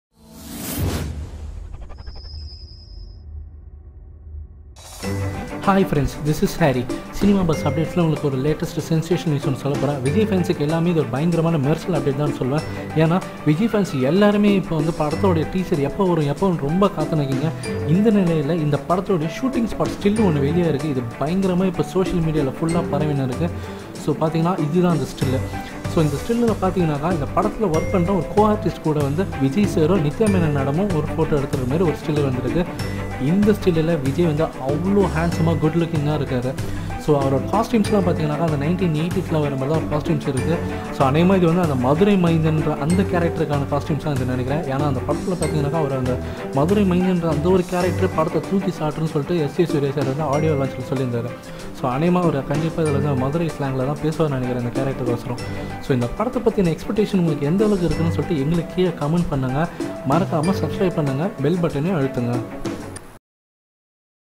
Trending! Ilayathalapathy Vijay Mersal Shooting Spot Still Goes Viral On Internet! || Kajal Agarwal
Mersal Movie Latest Shooting Spot Photo || Vijay | Mersal | Kajal Agarwal